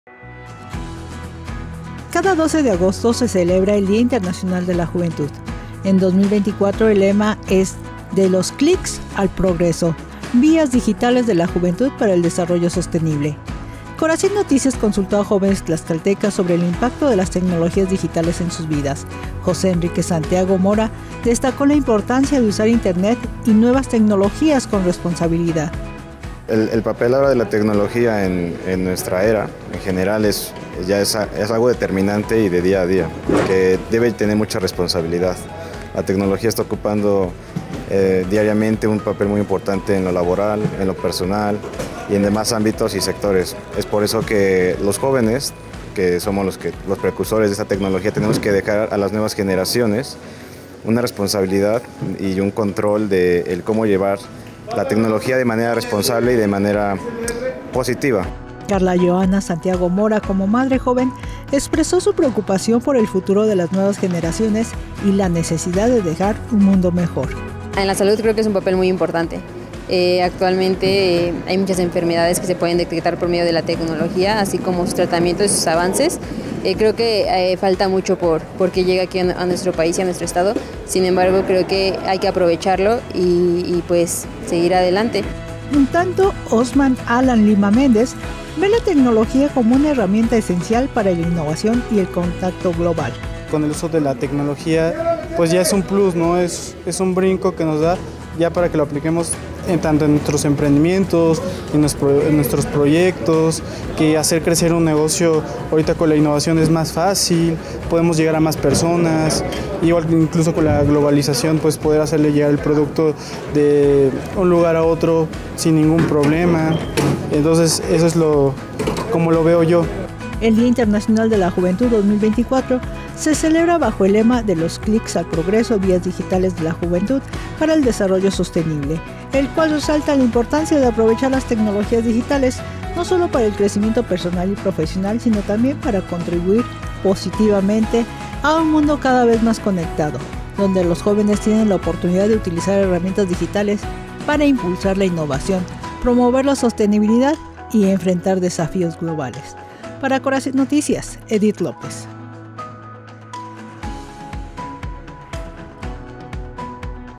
¿Cómo impacta la tecnología digital en la vida de los jóvenes?
Estas herramientas tienen una gran influencia a nivel social y emocional.